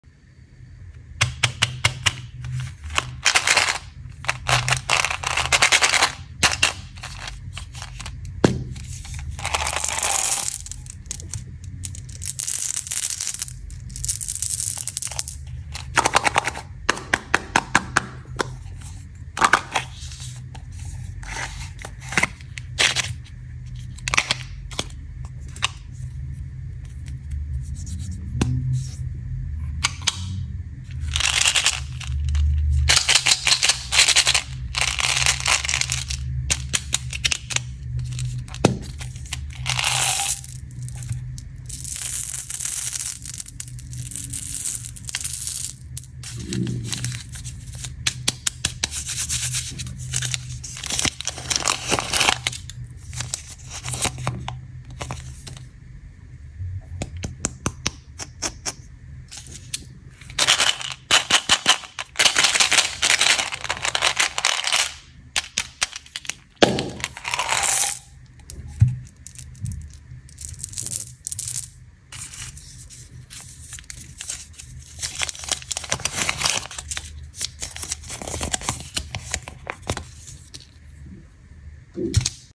How to unpack Rainbow Lollipop and Sweets Cutting | Lollipops Unpacking | ASMR | Satisfying Video
Relaxing Video with Yummy Lollipops. I love sweets, I hope you also love chocolate eggs or candy.